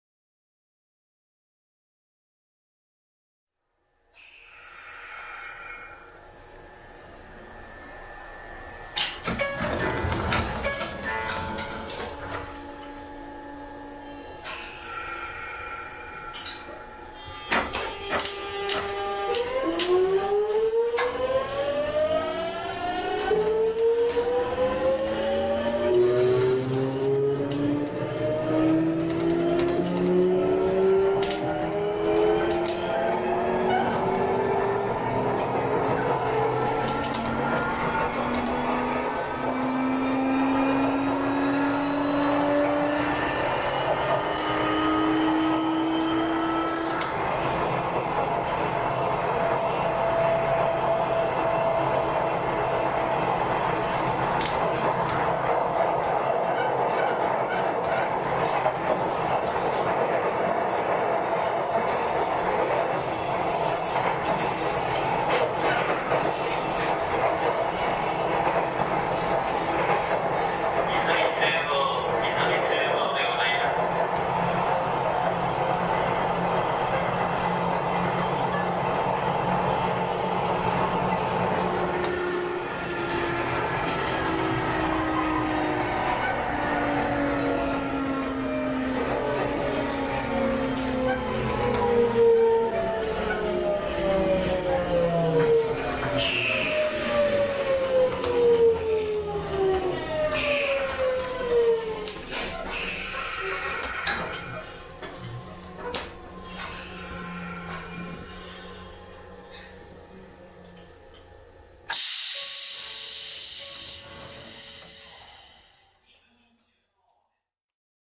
相鉄新7000系 走行音 Sotetsu Series New 7000 sound
相模鉄道 新7000系の走行音です。
制御装置：日立製 GTO-VVVF
録音区間：いずみ野線 各停 ゆめが丘→いずみ中央
※ご視聴の際は、音量にご注意下さい※
【相鉄新7000系について（Wikipedia）】

Sagami Railway
Series New 7000 sound
Izumino line local Yumegaoka→ Izumi-chūō
This video is sound only.
◇Please note the sound volume when you watch this video◇